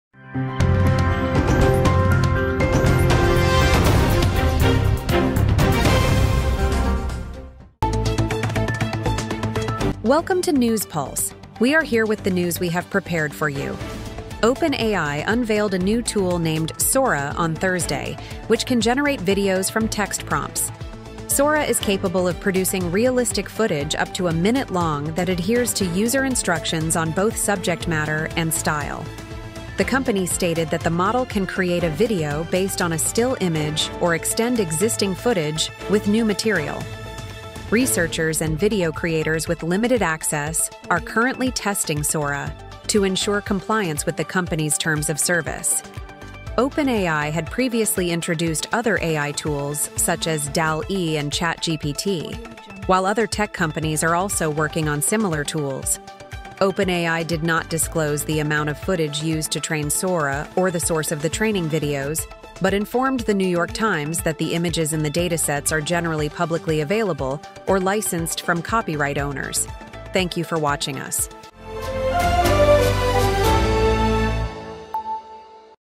OpenAI Unveils Sora Tool That Creates 1-Minute Realistic Videos from Text!
🌟🎥 OpenAI unveiled a groundbreaking tool called "Sora" on Thursday! Sora can generate videos from text prompts, creating realistic footage up to a minute long that follows user instructions on subject matter and style. 🚀

🎨 Sora can generate videos based on still images or extend existing footage with new material, staying true to user preferences.

Although OpenAI hasn't disclosed the amount of footage used to train Sora or its source, they mentioned to The New York Times that images in the datasets are generally publicly available or licensed from copyright owners.

🤖 OpenAI previously introduced other AI tools like Dall-E and ChatGPT. It's speculated that other tech companies are also working on similar tools.